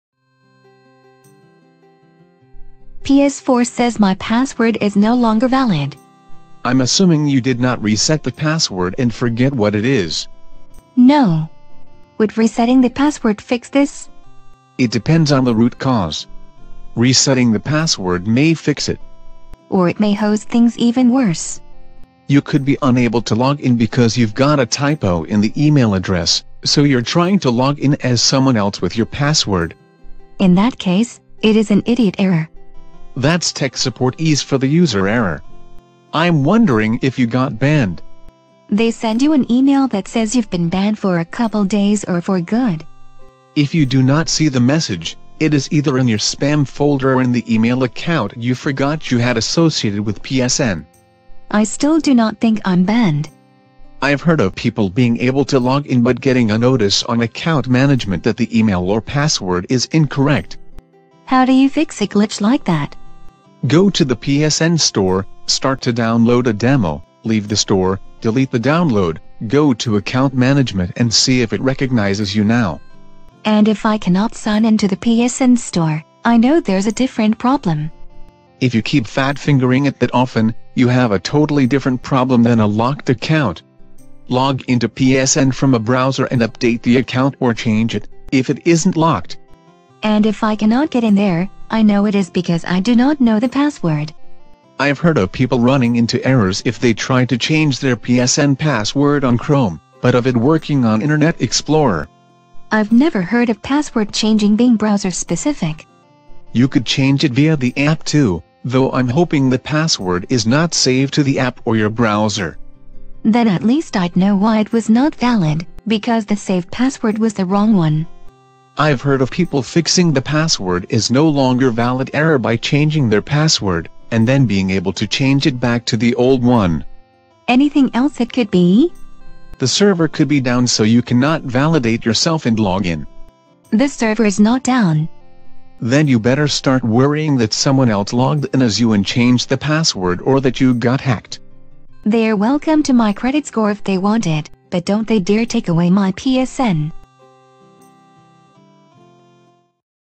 PS4 Says My Password is No Longer Valid
I’m assuming you did not reset the password and forget what it is.

No. Would resetting the password fix this?

It depends on the root cause. Resetting the password may fix it.

Or it may hose things even worse.

In that case, it is an ID10T error.

That’s tech support-ese for the user error. I’m wondering if you got banned.

They send you an email that says you’ve been banned for a couple days or for good.

If you do not see the message, it is either in your spam folder or in the email account you forgot you had associated with PSN.

I still do not think I’m banned.

I’ve heard of people being able to log in but getting a notice on account management that the email or password is incorrect.

How do you fix a glitch like that?

Go to the PSN store, start to download a demo, leave the store, delete the download, go to account management and see if it recognizes you now.

And if I cannot sign in to the PSN store, I know there’s a different problem.

If you keep fat fingering it that often, you have a totally different problem than a locked account. Log into PSN from a browser and update the account or change it, if it isn’t locked.

And if I cannot get in there, I know it is because I do not know the password.

I’ve heard of people running into errors if they try to change their PSN password on Chrome, but of it working on internet explorer.

I’ve never heard of password changing being browser specific.

You could change it via the app too, though I’m hoping the password is not saved to the app or your browser.

Then at least I’d know why it was not valid – because the saved password was the wrong one.

I’ve heard of people fixing the password is no longer valid error by changing their password, and then being able to change it back to the old one.

Anything else it could be?

The server could be down so you cannot validate your self and log in.

The server is not down.

Then you better start worrying that someone else logged in as you and changed the password or that you got hacked.

They are welcome to my credit score if they want it, but don’t they dare take away my PSN.